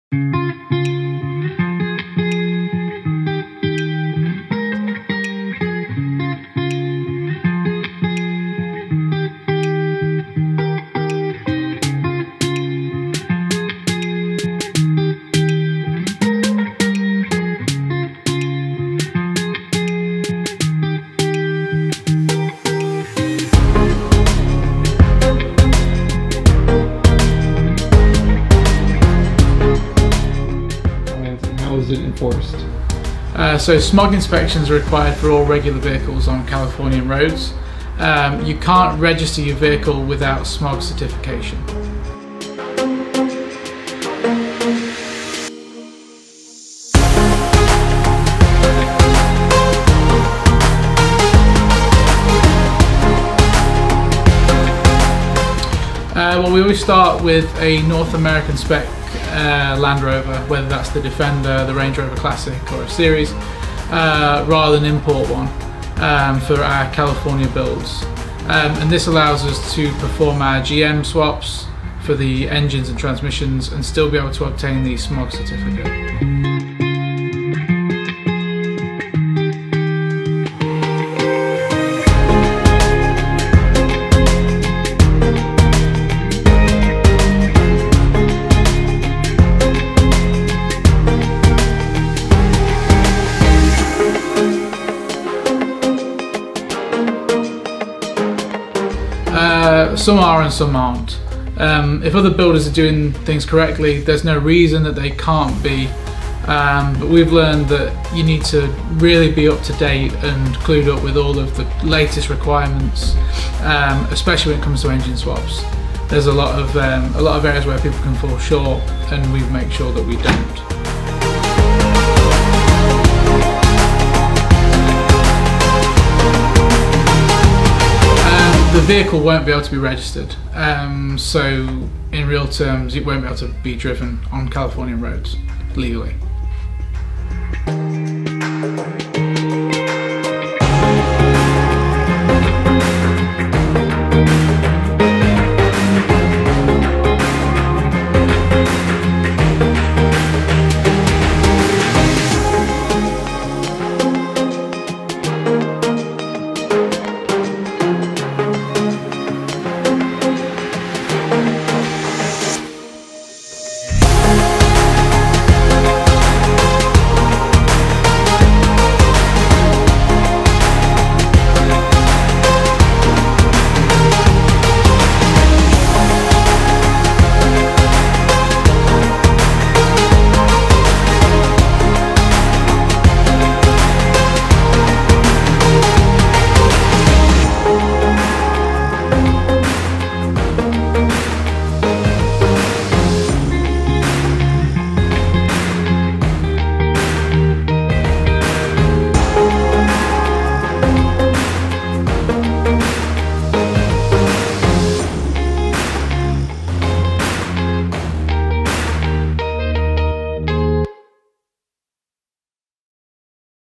Making Your Defender California Compliant | "Behind the Build" EP. 26 | E.C.D. Automotive Design
Here is the link for more information from ECD about California Compliance!